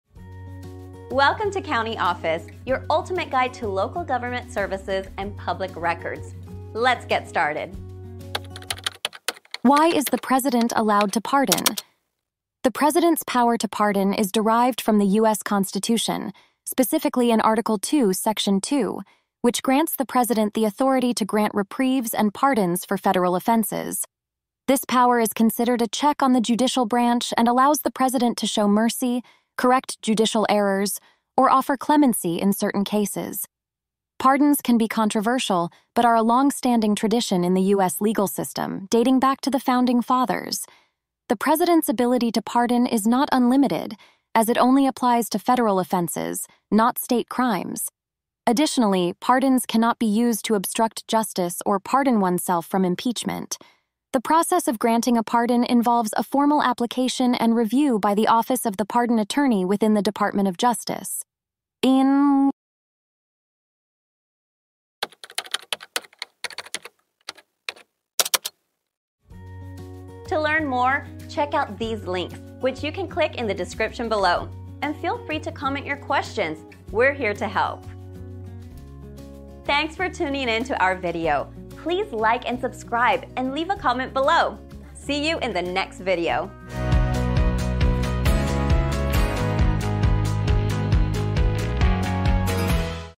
Why Is The President Allowed To Pardon? - CountyOffice.org
Why Is The President Allowed To Pardon? In this enlightening video, we delve into the intriguing topic of the President's power to pardon, exploring its constitutional basis and historical significance in the U.S. legal system. The authority to grant reprieves and pardons for federal offenses, as outlined in Article II, Section 2 of the Constitution, serves as a crucial tool for the President to exercise mercy, rectify judicial errors, and offer clemency in specific cases. While pardons have sparked debates, they remain a time-honored tradition since the days of the Founding Fathers. It's essential to understand that the President's pardon authority has limitations, such as only applying to federal crimes and not encompassing state offenses or the ability to pardon oneself from impeachment. The intricate process of granting a pardon involves formal application and meticulous review by the Office of the Pardon Attorney within the Department of Justice.